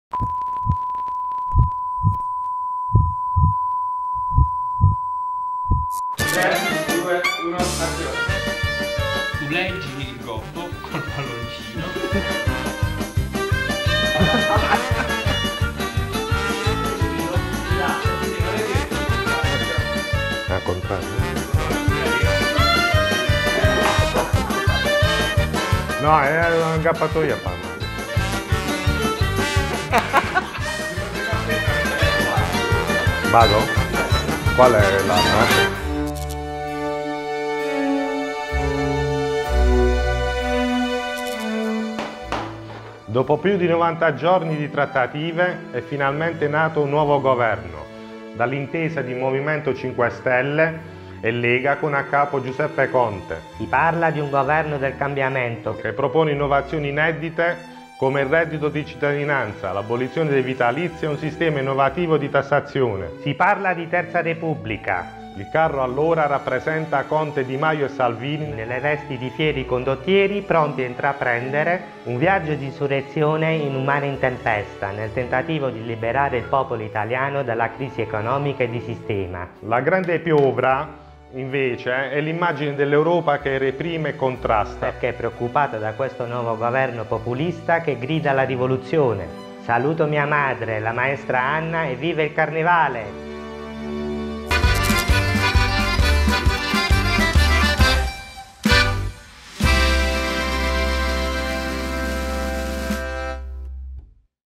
CARNEVALE DI PUTIGNANO 2019 | CARRISTI ALLO SBARAGLIO | LIPPOLIS
🎉🎭 CARRISTI ALLO SBARAGLIO 🎺🎉
I 7 maestri cartapestai hanno provato a spiegarci i bozzetti dei loro carri, ma sono usciti pazzi.. "Il maestro e l'alunno", è il quarto racconto del carrista allo sbaraglio affetto da problemi di doppia personalità, la sentite anche voi quella strana vocina? 😜🤣

Carrista Pazzo: Francesco Lippolis
Titolo carro: "La Revolution Italienne"
a cura di: ass. Carta Bianca
Una pazza produzione video: Traipler.com

🎭 ✨Esci pazzo e vieni a scherazare in Puglia alle sfilate del CarnevaleDiPutignano, acquista il biglietto online, salta la fila e sfila